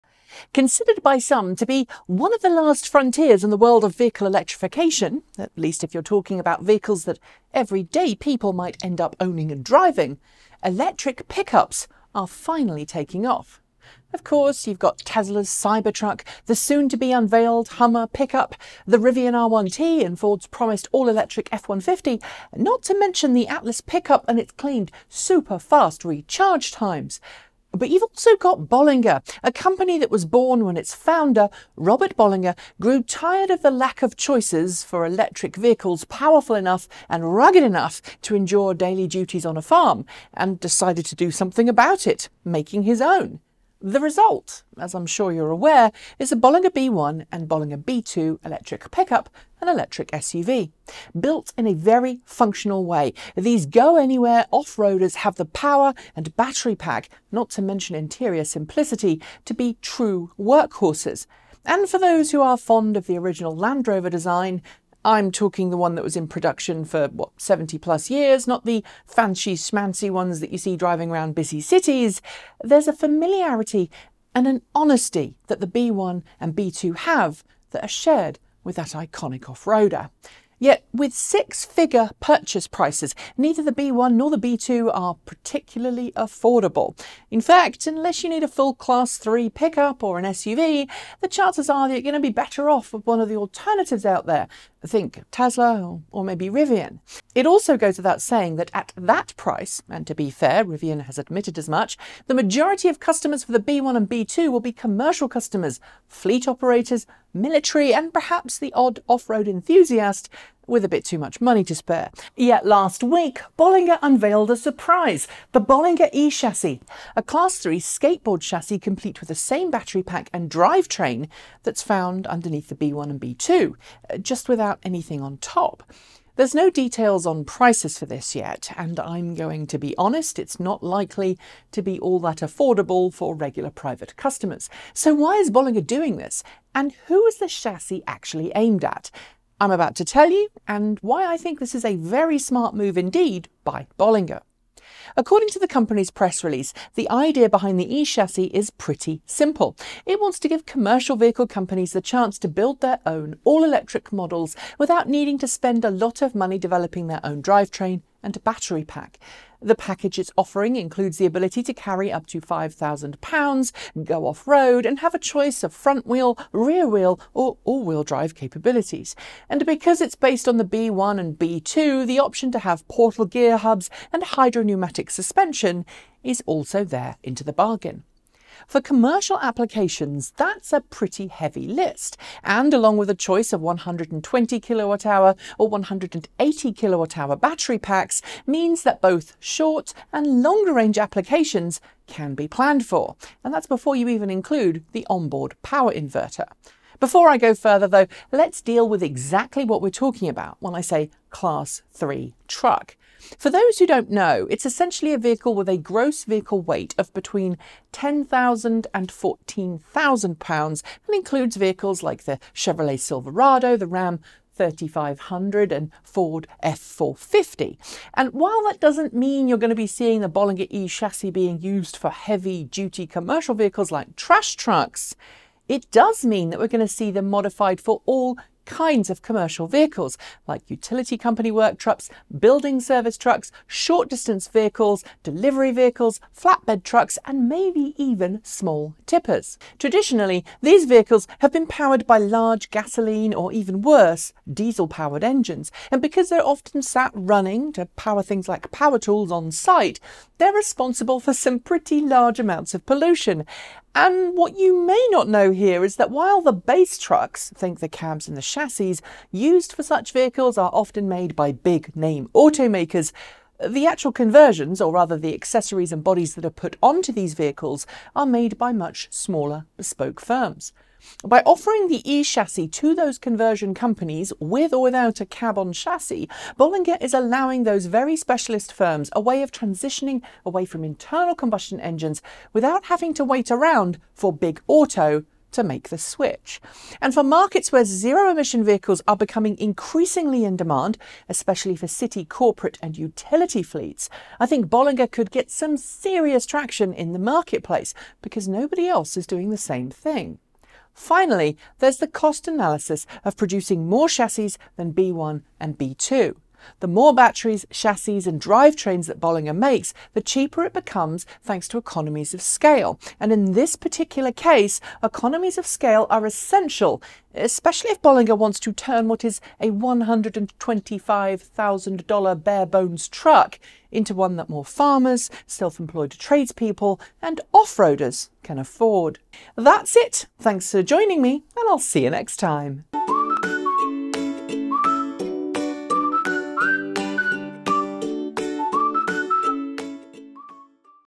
Why This Truck Company Is Making Bare Chassis!
Specialist trucks -- used in construction, maintenance and of course, by utility companies -- are often built by specialists based off existing internal combustion engine bodies.

But now in the U.S., Bollinger is looking to change that by offering conversion specialists an all-electric chassis on which to build their commercial vehicles.

NZs ONLY 100% Renewable carboNZero Certified Electricity
100% Kiwi % 50% Community Owned

And we're powering the Aotearoa electric vehicle future!